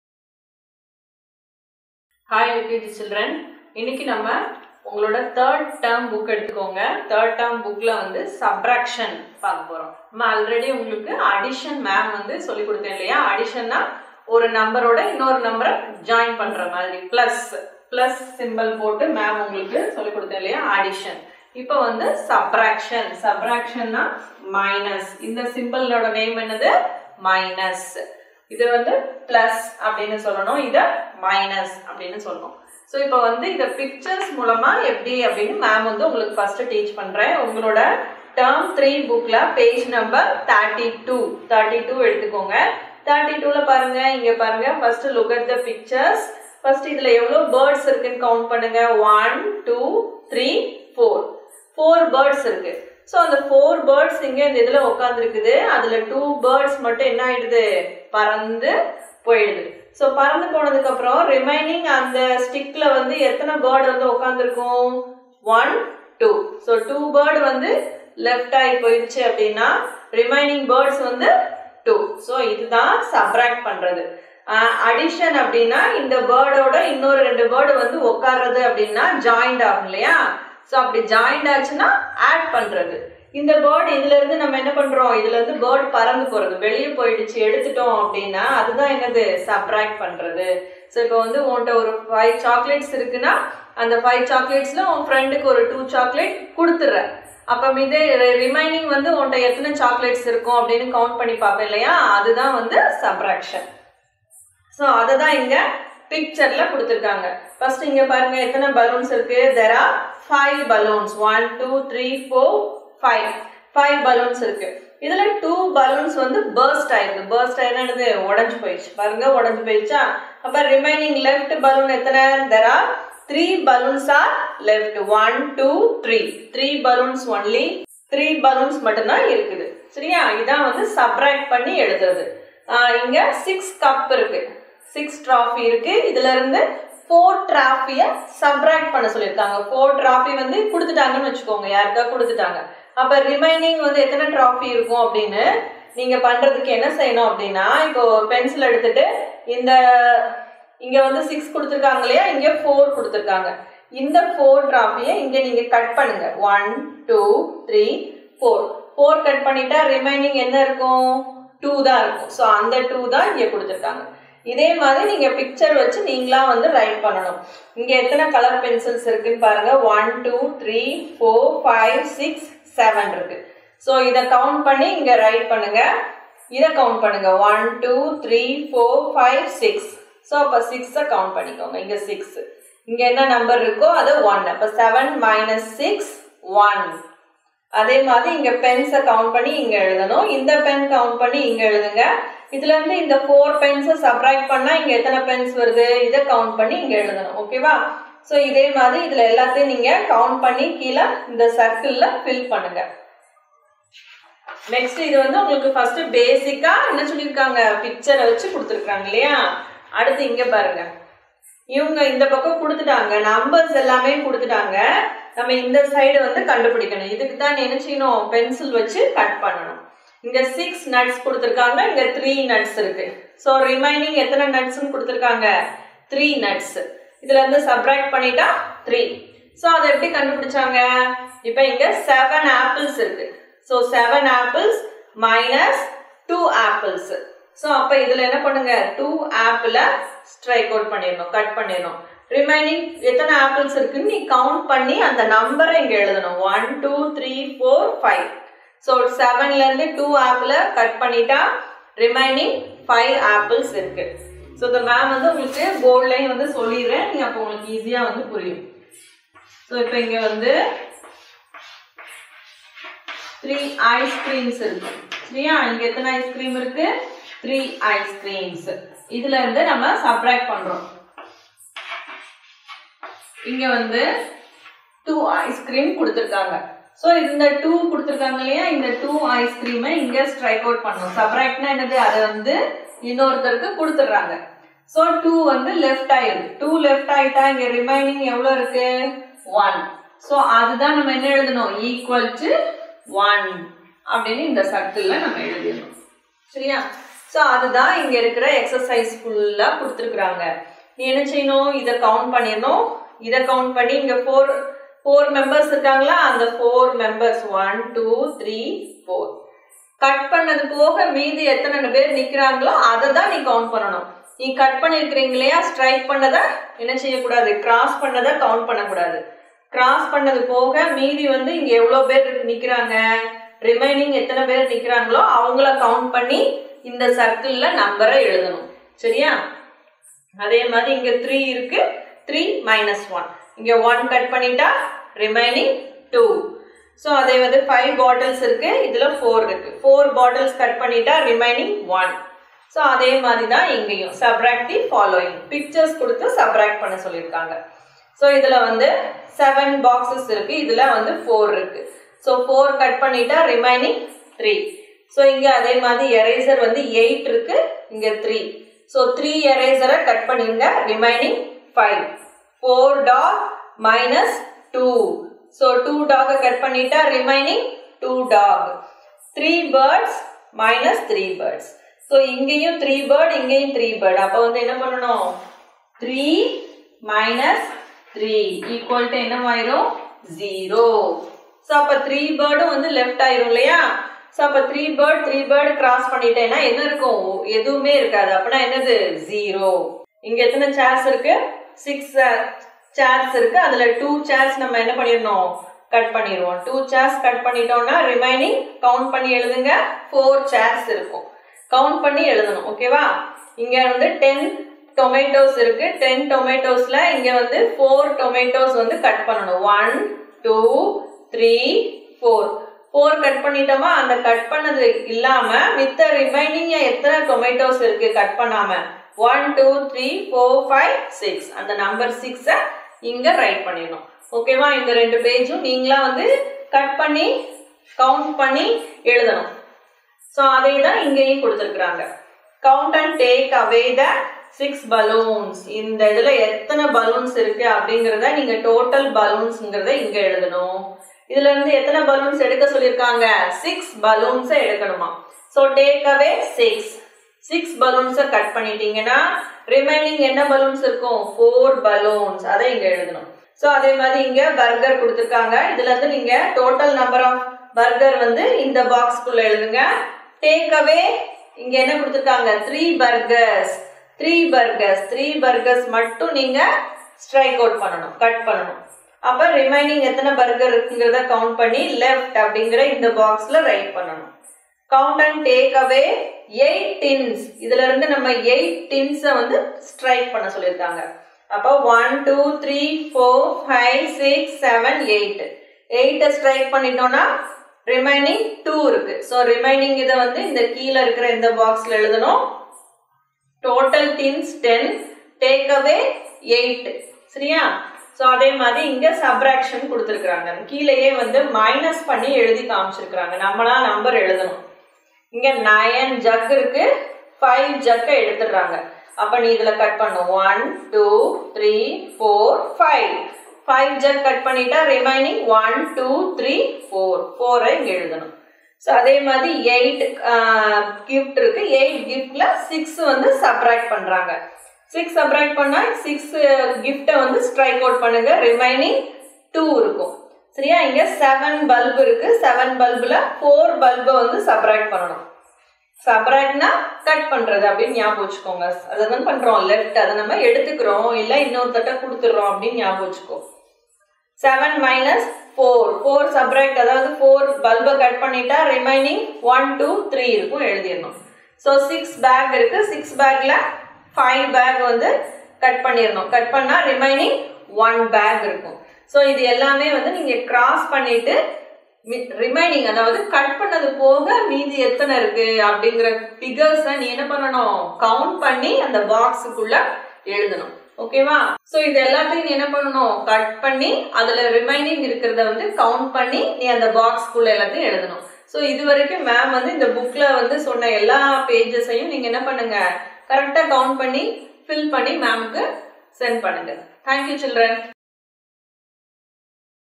Mathematics - Subtraction (Part 1) l Term 3 Book Page No. 32 to 37 for UKG Kids
KIDDIES KINGDOM NURSERY SCHOOL
Krishna Nagar, Kovilpatti